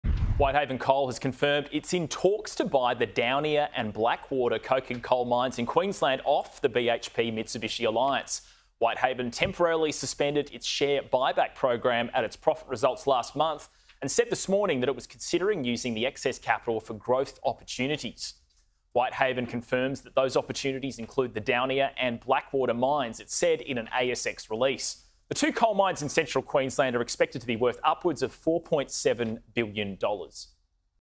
Whitehaven coal confirms talks of buying two Queensland coal mines
Whitehaven Coal has confirmed it is in talks to buy the Daunia and Blackwater coking coal mines in Queensland off the BHP Mitsubishi Alliance.

Whitehaven temporarily suspended its share buy-back program at its profit results last month.

It said on Monday morning it was considering using the excess capital for growth opportunities.

In an ASX release, they said “Whitehaven confirms that those opportunities include the Daunia and Blackwater mines.”

The two coal mines in central Queensland are expected to be worth upwards of $4.7 billion.